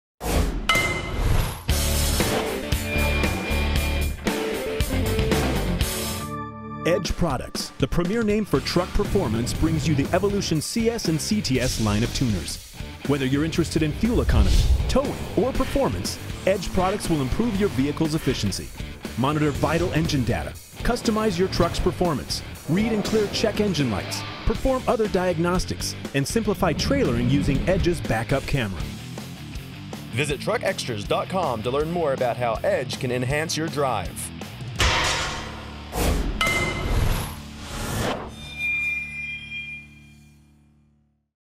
Introducing the most advanced Edge Evolution CS and CTS ever!
Introducing the most advanced Edge Evolution CS and CTS ever! With all-new features the Evolution offers the industry-leading features and custom options! The Evolution CS/CTS comes with multiple power levels and installs in minutes, without ever popping the hood. The Evolution CS/CTS increases power up to 100 hp and 200 ft-lbs of rear wheel torque. Not only does it re-tune your vehicle, it stays mounted in the cab and features real-time, in-cab monitoring of vital engine data. It can also be used as a diagnostic scan tool to read and clear "check engine" lights and diagnostic trouble codes.